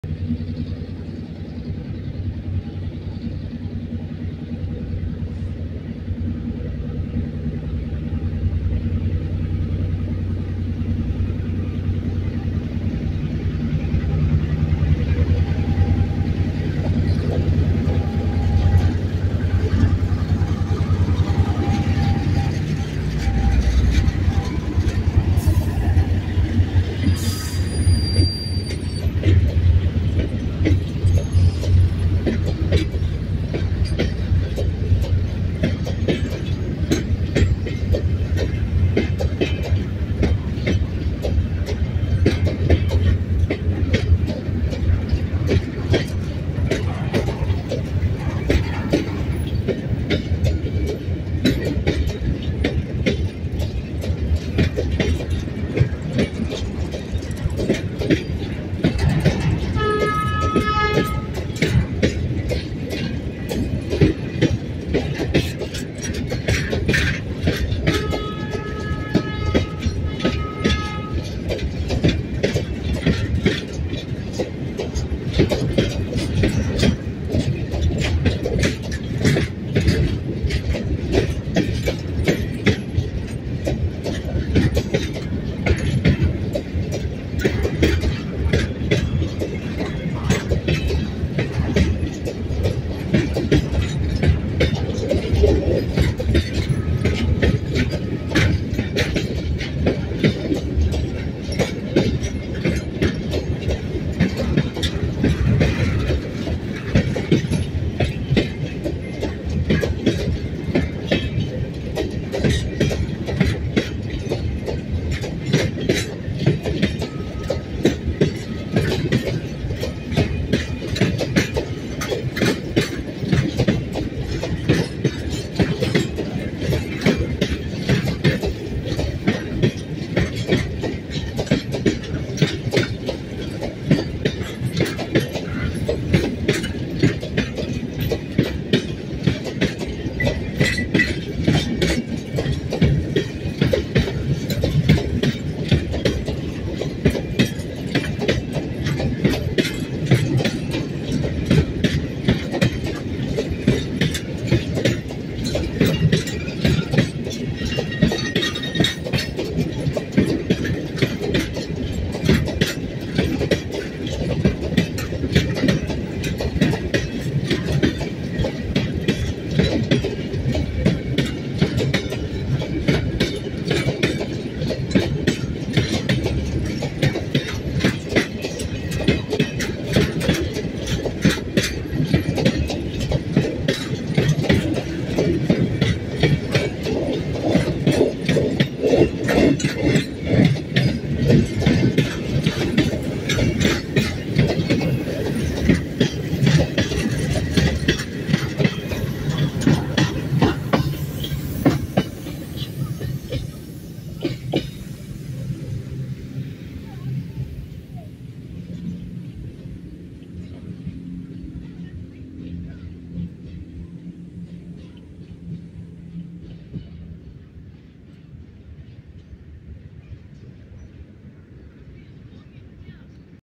NCA- Vacío saliendo de Villa María a cargo de las locomotoras GR12W 6622+ALCO RSD-16 8242
Luego de maniobrar, las locomotoras GR12W 6622 y ALCO RSD-16 8242 hacen su salida de Villa María con un tren vacío rumbo a General Deheza. 09/08 18:53 PM.